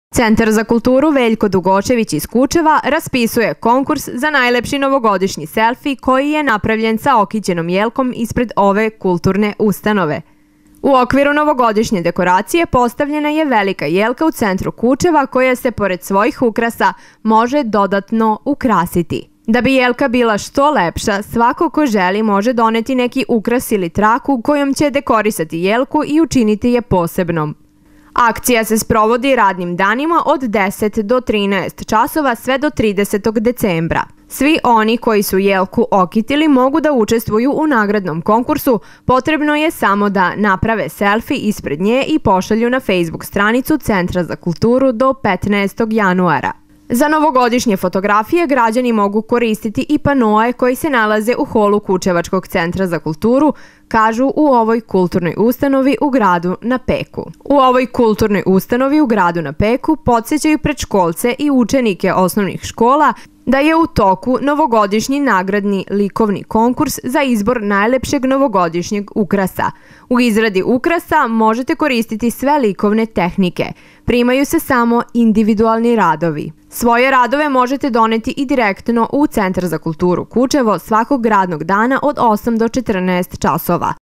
Građani Kučeva sami ukrašavaju jelku i slikaju, E Kučevo (RTV MLAVA 17.12.2020.)
Lj. Nastasijević